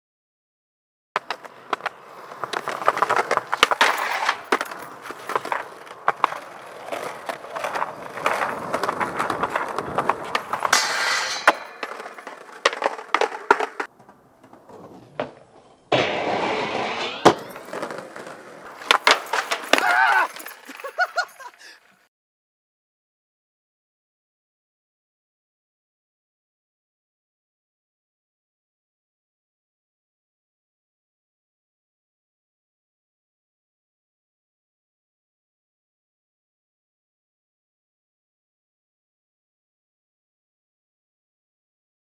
Wes Fleming Line in the Okanagan
Wes throws down with a heavy line at a popular skate spot in the Okanagan. A half-cab Bs Crooked grind, followed up by Wes's world-famous Bs 50-50.